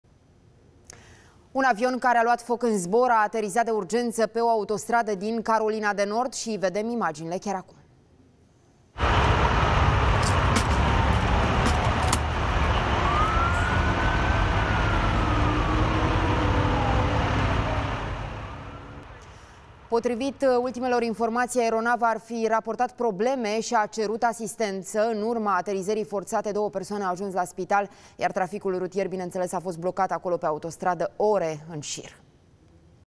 Un avion cuprins de flăcări a aterizat forțat pe autostradă
Un avion care a luat foc în zbor a aterizat de urgență pe o autostradă din Carolina de Nord.